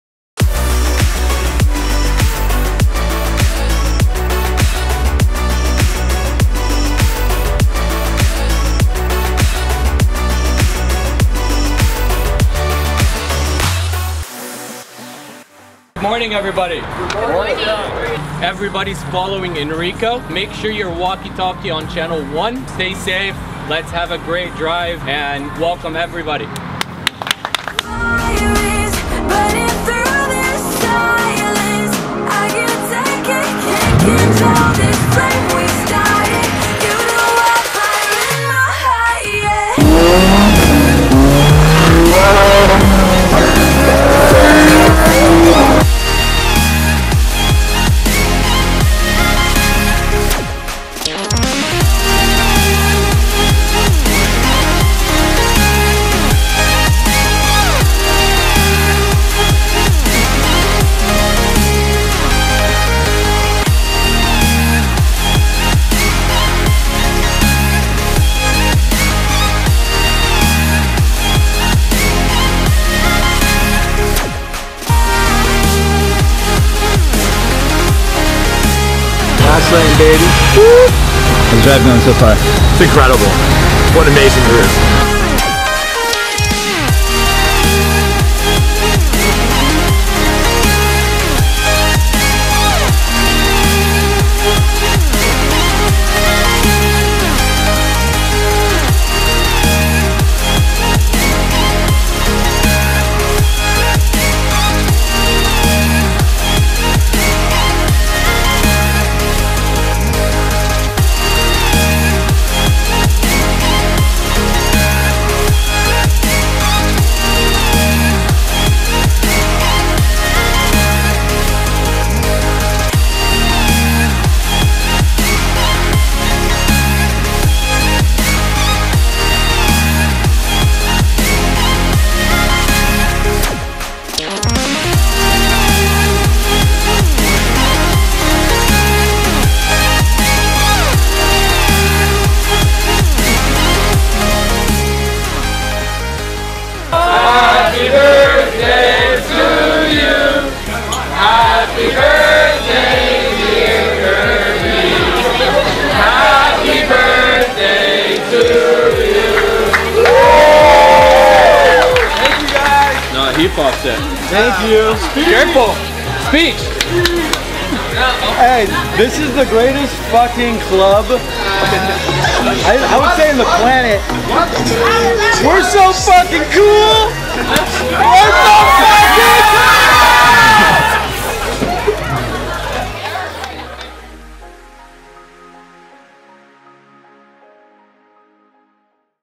Lamborghini Performante & McLaren 720S - Fast Lane Drive Club Rally Pacific Beach | Car Club
Welcome to our new video series about Fast Lane Drive members! We're are going to introduce to you what Fast Lane Drive is about: the kind of work our members put in that has allowed them to drive their dream cars, and what motivates them to continue. We'll dive into the person behind the vehicle, their successes, and failures. Lastly, in today's video, you will meet our member who owns the first Lamborghini Huracan Spyder delivered in the United States.